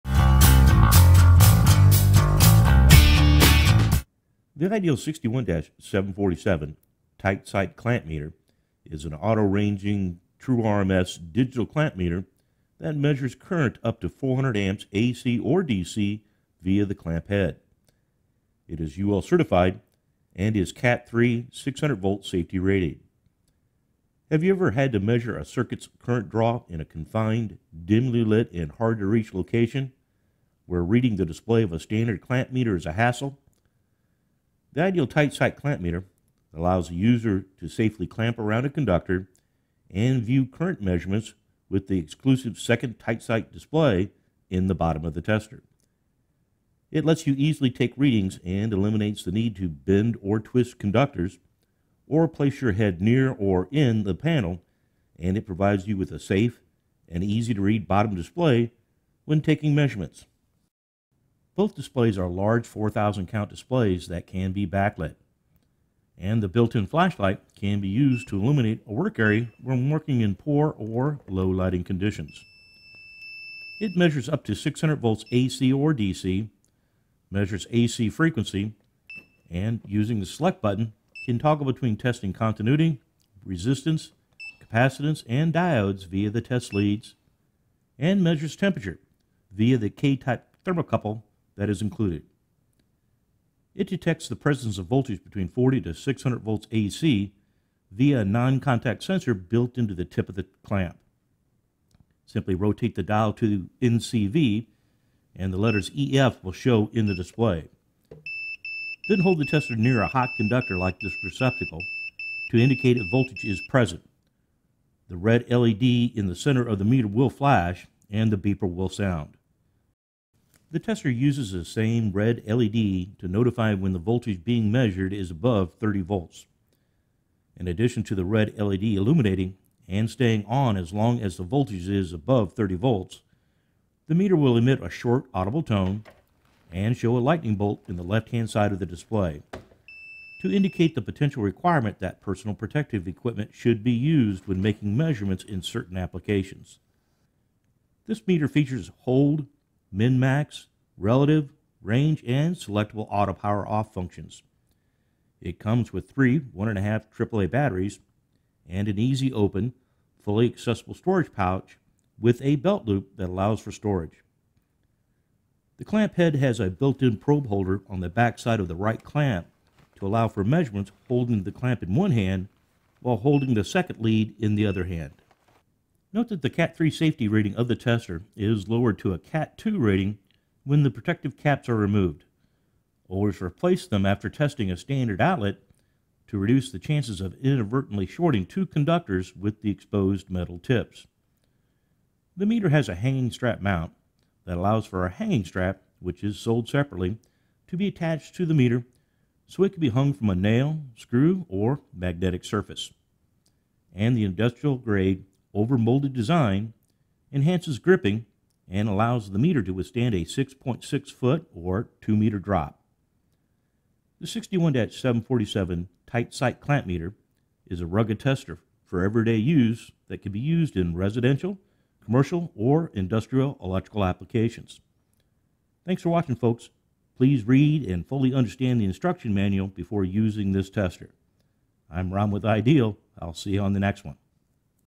How-To Use the IDEAL Electric 61-747 400A TightSight Clamp Meter
Join Ron Kipper, IDEAL’s product expert, in learning about the product features and how-tos of the IDEAL Electric 61-747 Clamp Meter. This clamp meter features TightSight and is an auto ranging true RMS digital clamp meter that measures current up to 400 amps AC or DC via the clamp head. It is UL Certified and CAT III 600 volt safety rated. Want to learn more about the 61-747?

The IDEAL 61-747 TightSight Clamp Meter is an auto ranging true RMS digital clamp meter that measures current up to 400 Amps AC or DC via the clamp head.
It is UL certified and is CAT III 600 volt safety rated.
Have you ever had to measure a circuits current draw in a confined, dimly lit and hard to reach location where reading the display of a standard clamp meter is a hassle? The IDEAL TightSight Clamp Meter allows the user to safely clamp around a conductor and view current measurements with the exclusive second TightSight display in the bottom of the tester. It lets you easily take readings and eliminates the need to bend or twist conductors or place your head near or in the panel and it provides you with a safe and easy-to-read bottom display when taking measurements.
Both displays are large 4000 count displays that can be backlit, and the built-in flashlight can be used to illuminate a work area when working in poor or low lighting conditions.
It measures up to 600 volts AC or DC and frequency and using the select button can toggle between testing continuity, resistance, capacitance, and diodes via the test-leads and measures temperature via a K-Type thermocouple that is included.
It detects the presence of voltage between 40 to 600 volts AC via a non-contact sensor built into the tip of the clamp. Simply rotate the dial to NCV and the letters EF will show in the display. Then hold the tester near a hot conductor like this receptacle to indicate if voltage is present, the red LED in the center of the meter will flash and the beeper will sound.
The tester uses the same red LED to notify you when the voltage being measured is above 30 volts.  In addition to the red light illuminating and staying  on as long as the voltage is above 30 volts, the meter will emit a short audible tone, and show a lightning bolt in the left hand side of the display to indicate the potential requirement that personal protective equipment should be used when making measurements in certain applications.
This meter features HOLD, MIN/MAX, Relative, Range, and selectable Auto Power Off functions. It comes with three 1.5V AAA Batteries and an easy open, fully accessible storage pouch with a belt loop that allows for storage.
The clamp head has a built-in probe tip holder on the backside of the right clamp to allow measurements holding the clamp in one hand while holding the second lead in the other hand.
Note that the CAT III safety rating of the tester is lowered to a CAT II rating when the protective caps are removed. Always replace them after testing a standard outlet to reduce the chances of inadvertently shorting two conductors with exposed metal tips.
The meter has a hanging strap mount that allows for a hanging strap which is sold separately to be attached to the meter so it can be hung from a nail, screw, or magnetic surface.  And the industrial grade over molding enhances gripping and allows the meter to withstand a 6.6-foot (2 m) drop.
The 61-747 TightSight Clamp Meter is a rugged tester for everyday use that can be used in residential, commercial, or industrial electrical applications.